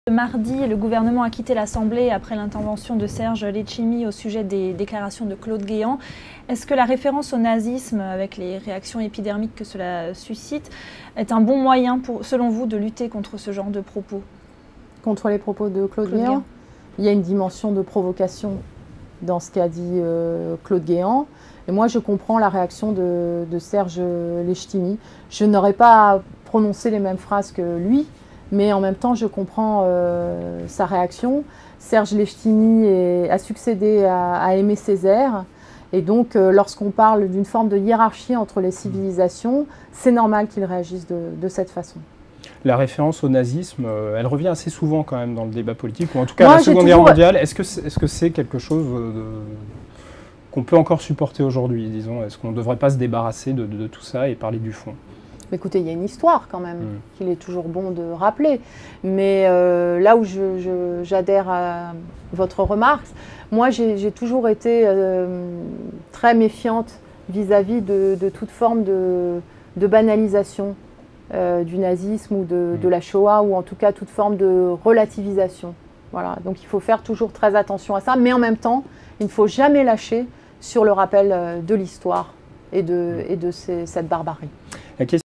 Delphine Batho - Letchimy contre Guéant : "Je n'aurais sans doute pas utilisé ces propos"
La députée socialiste des Deux-Sèvres, porte-parole de François Hollande pour l'élection présidentielle, était l'invitée de l'Emission politique de L'Internaute.